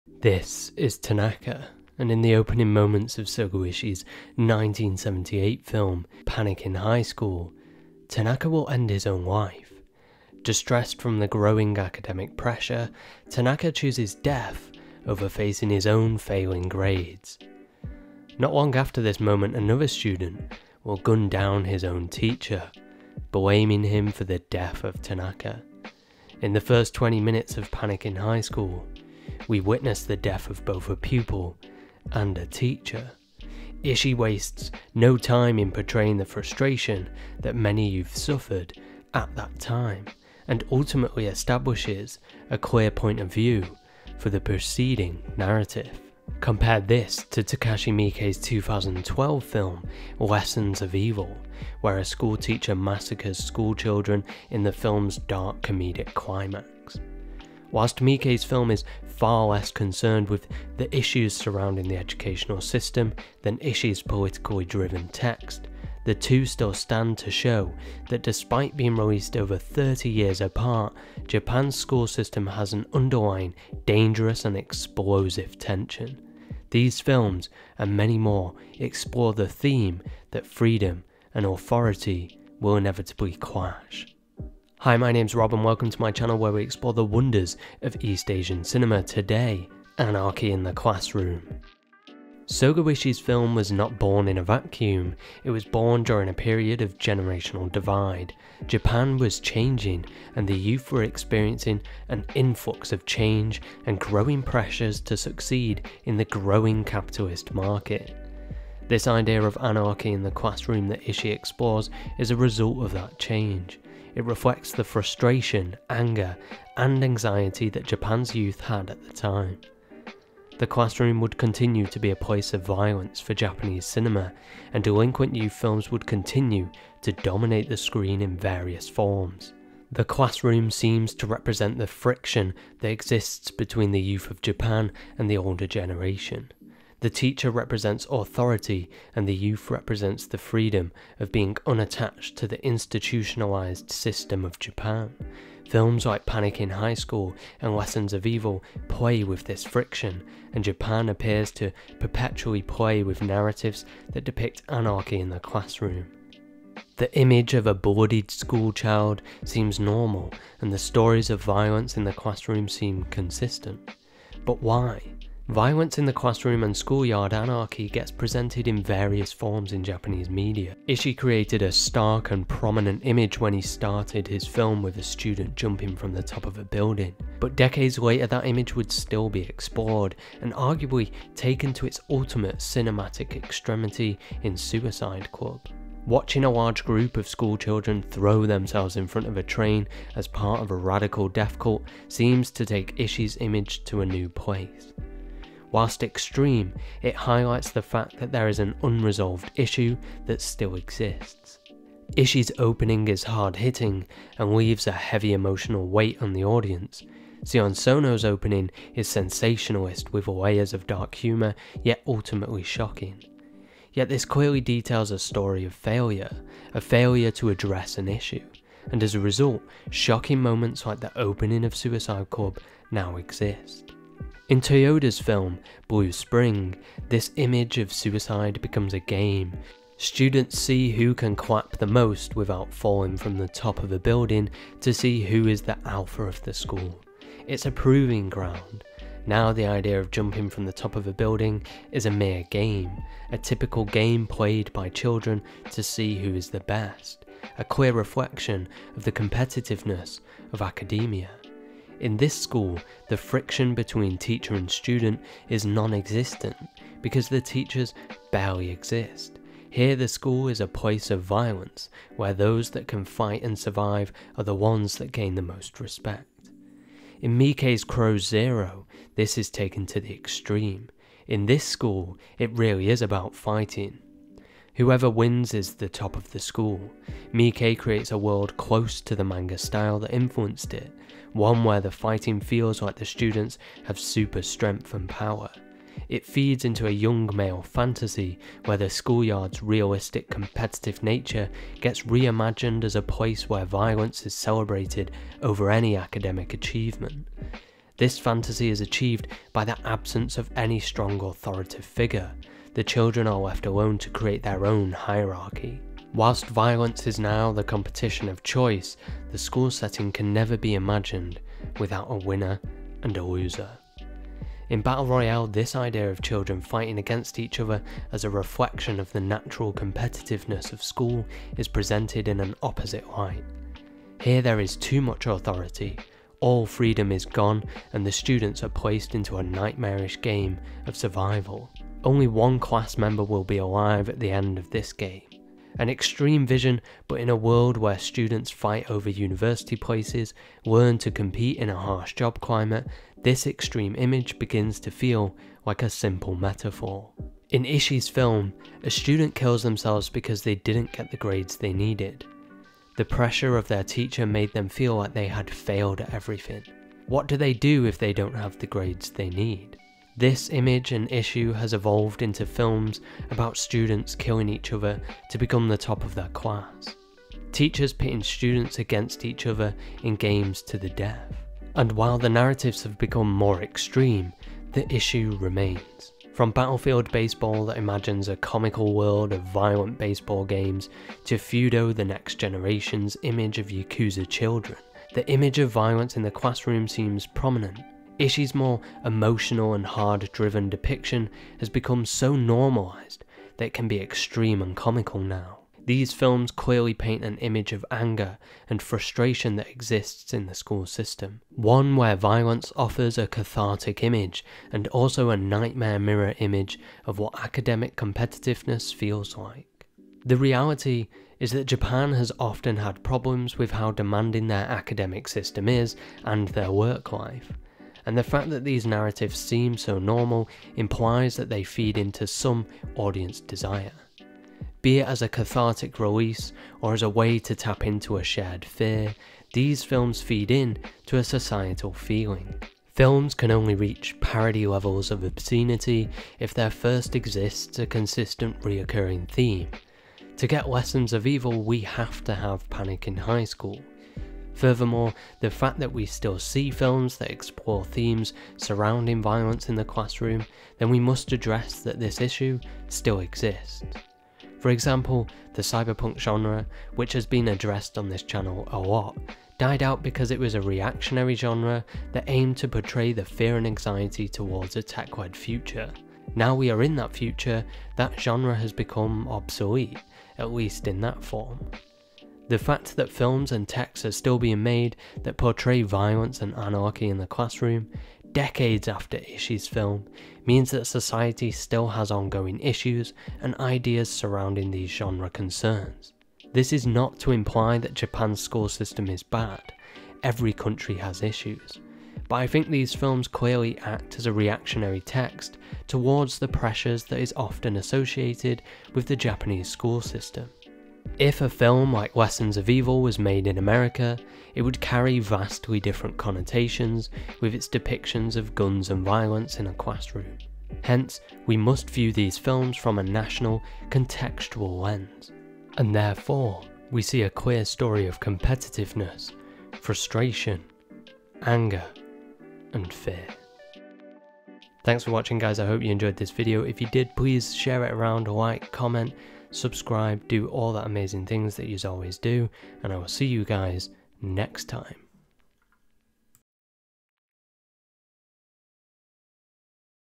Japan and violence in the classroom: Panic in Highschool, Battle Royale, Crows Zero, and much more.
Japan has a history of representing visions of violence in the classroom. From Sogo Ishii's Panic in Highschool to Takashi Miike's Lessons of Evil, the image of school students being violent always appears, but why? In this video we take a look at some of the films such as Crows Zero, Blue Spring, Suicide Club, Battle Royale, and many more, to see why this genre is so popular.